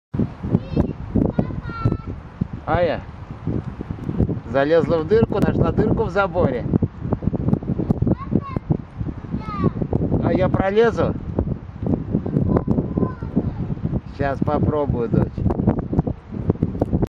11-19-2011 - Северный речной вокзал-2
Москва, 2011

гуляли с дочкой Евой по Северному речному вокзалу...